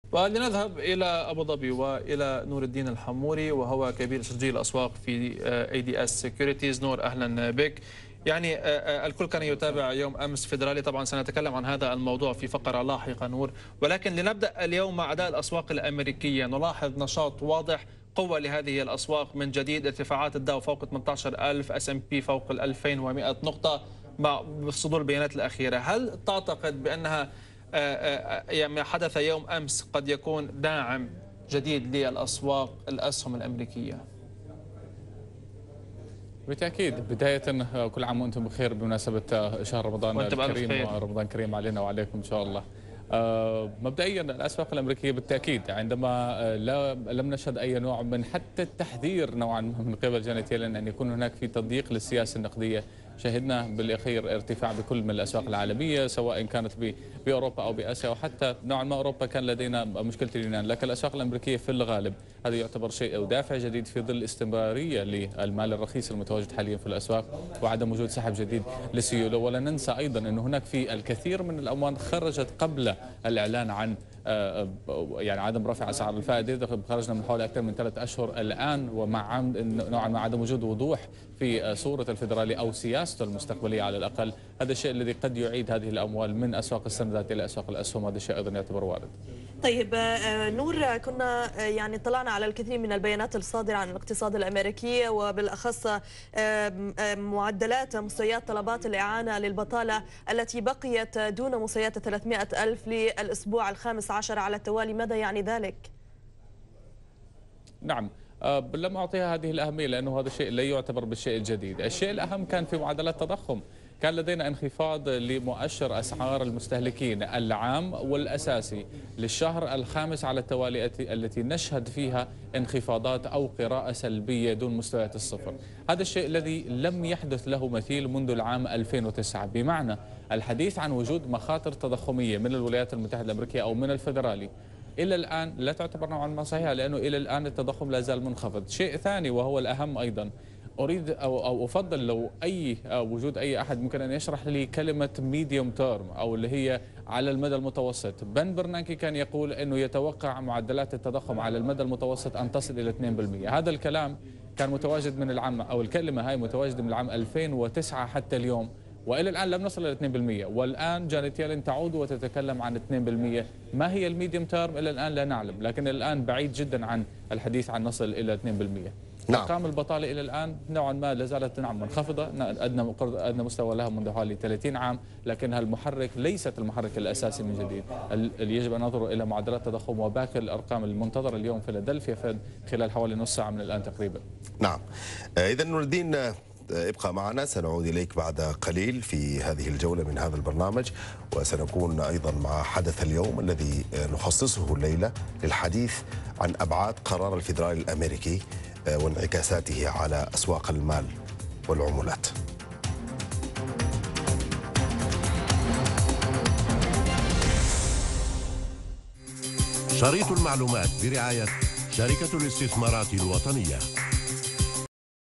Nour Eldeen on CNBC Arabia 18.June.2015 Part 1
Discussing the Federal Reserve decision, Janet Yellen press conference, USD trends and the Greek negotiations